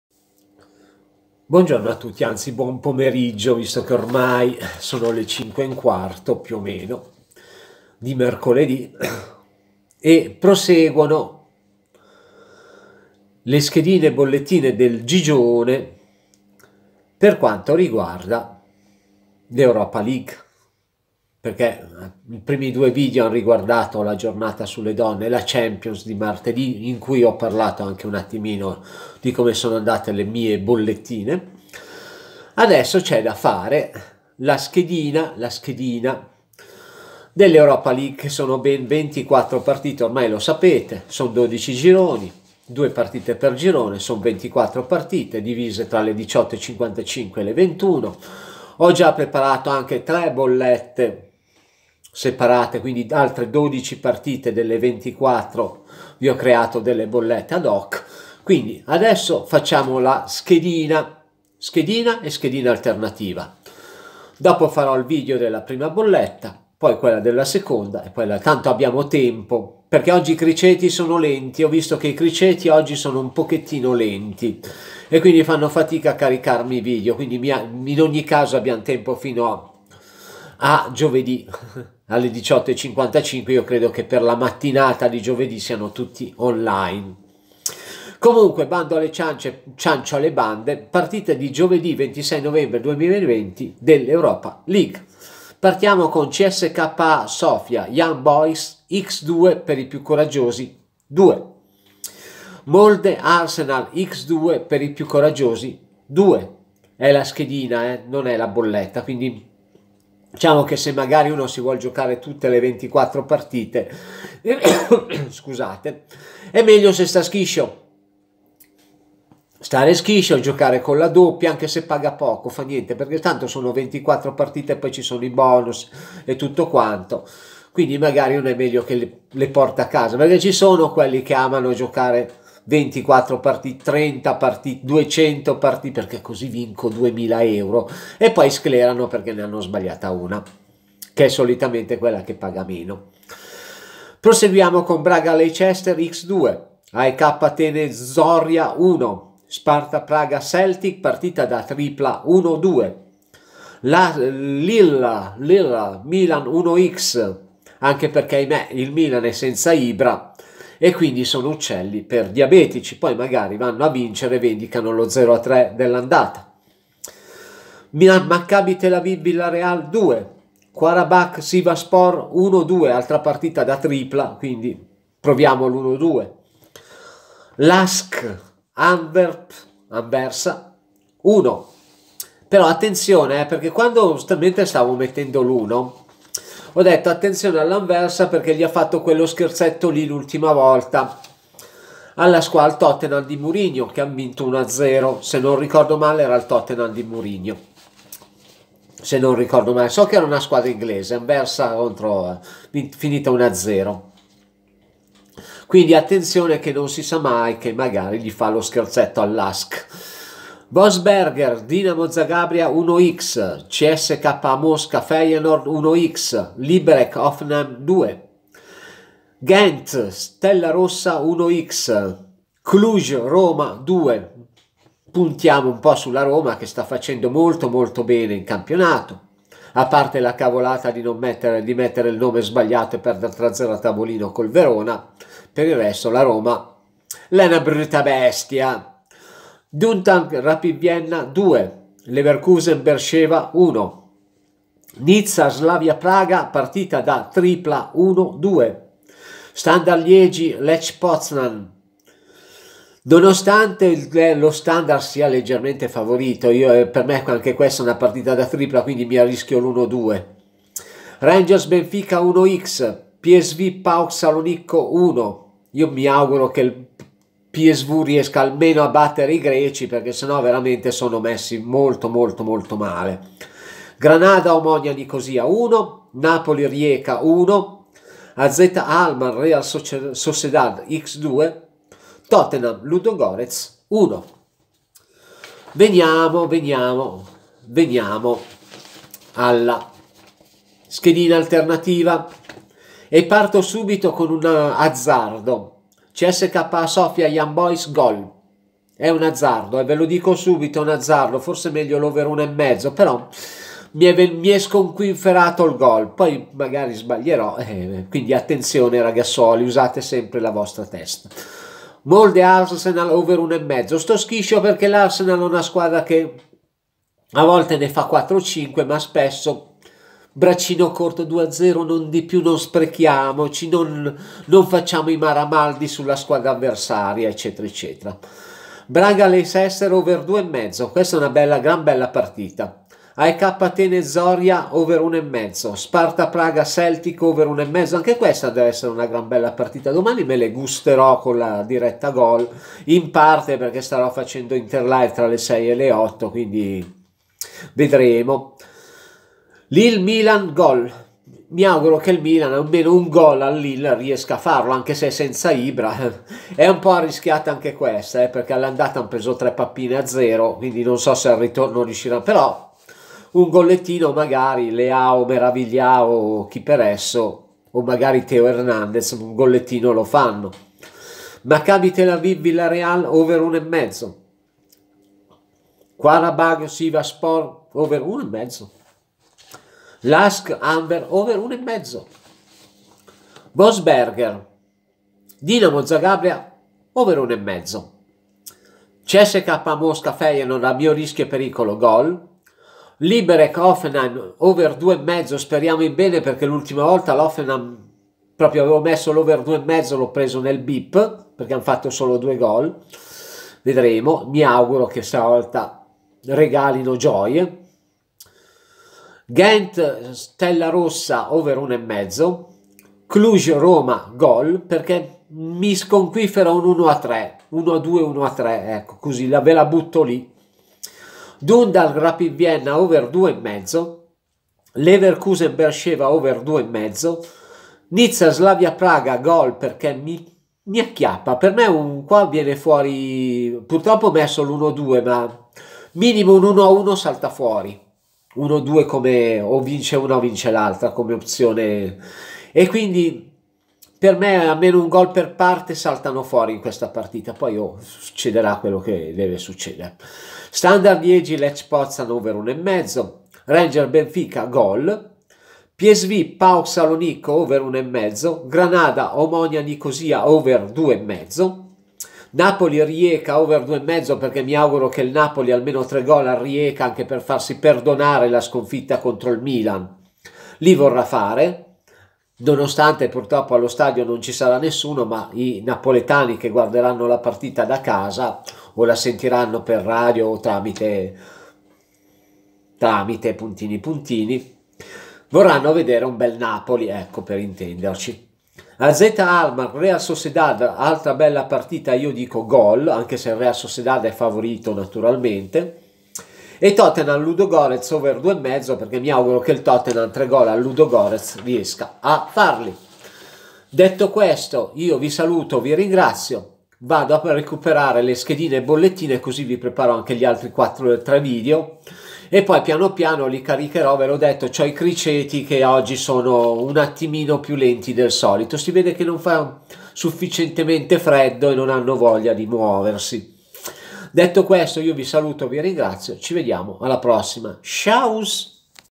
Europa League le schedine del Gigione per il 26 novembre 2020
Come sempre quando c'è l'Europa League super schedine con i pronostici di tutte le 24 partite e anche con l'alternativa